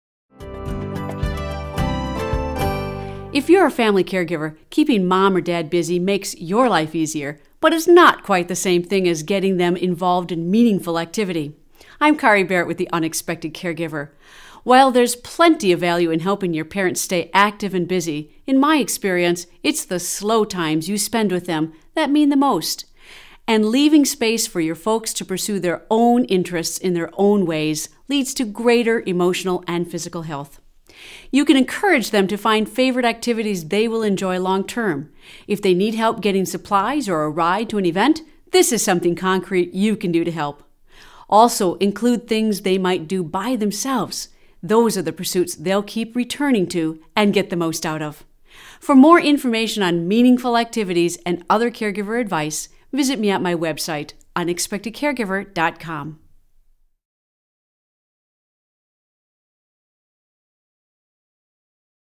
Caregiver Minute: Busy vs. Engaged
Quiet times are just as important, if not more so, than busy times